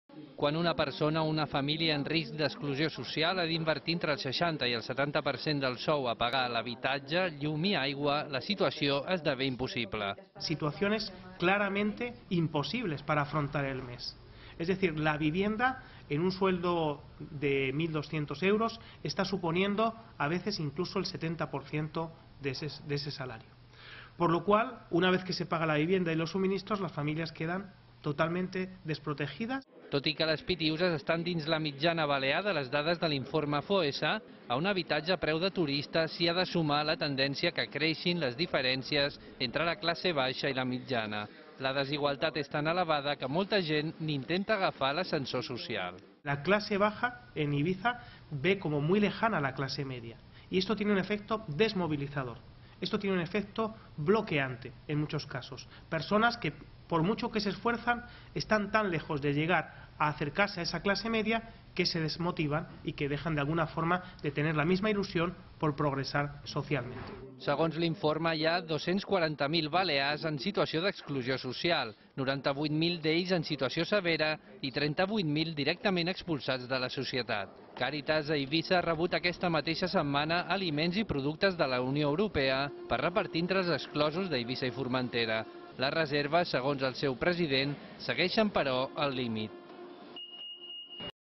La desigualtat social i l’habitatge accentuen les diferències entre els habitants de les Pitiüses
L’illa d’Eivissa pateix la desigualtat de renda més notable i creixent de totes les Balears. Ho ha dit aquest dimarts Raúl Flores, director tècnic de Foessa i coordinador de l’estudi de Caritas. Una situació d’exclusió social que, mesclada amb el difícil accés a l’habitage, dibuixen un panorama molt preocupant per a l’ONG.

Segons l’informe, hi ha 240.000 balears en situació d’exclusió social, 98.000 d’ells en situació severa i 38.000 directament expulsats de la societat. Caritas ha rebut aquesta mateixa setmana aliments i productes de la Unió Europea per repartir entre els exclosos d’Eivissa i Formentera. Les reserves, segons el seu president, Joan Marí, segueixen, però, “al límit”.